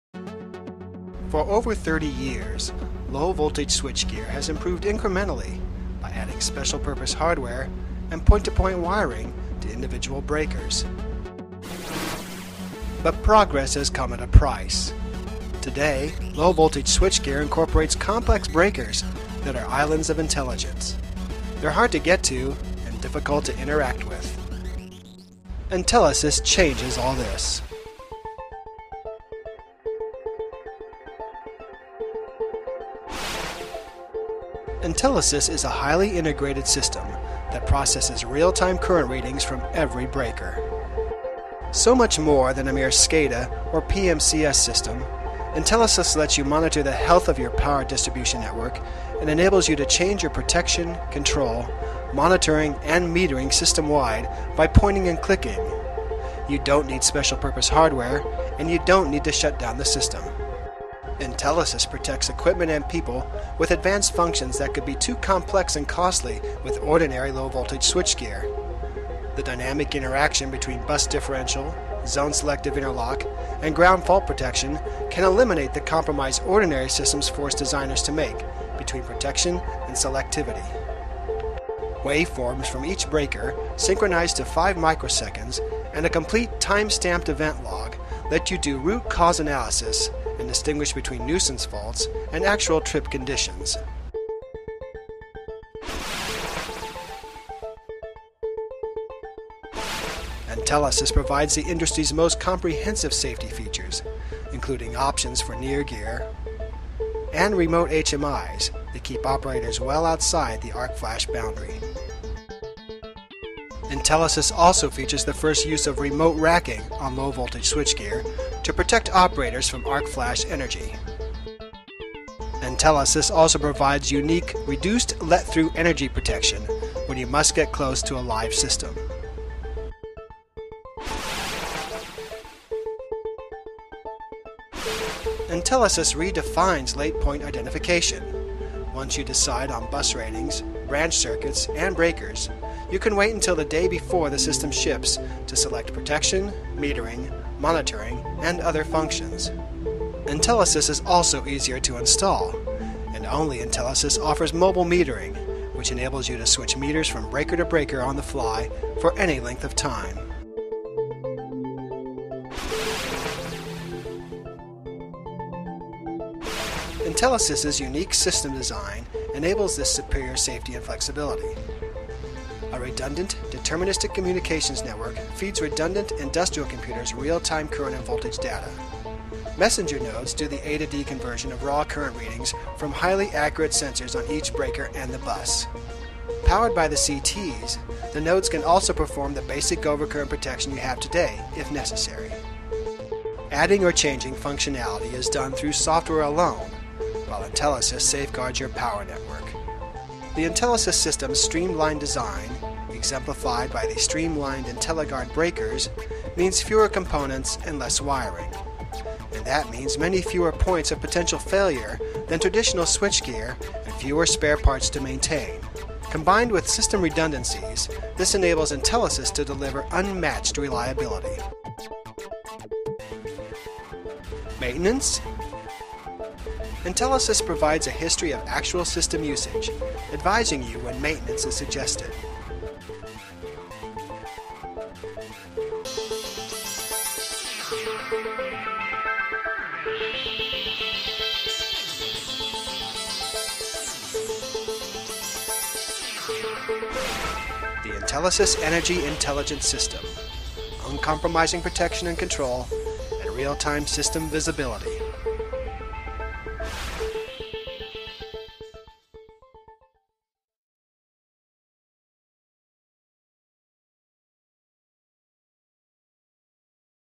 GE Entellisys Switchgear Offers Unique Arc Flash Protection Options
GE Entellisys Switchgear offers unique safety options allowing operators to perform all protection, control, monitoring, and diagnostics from remote locations, well outside the Flash Protection Boundary.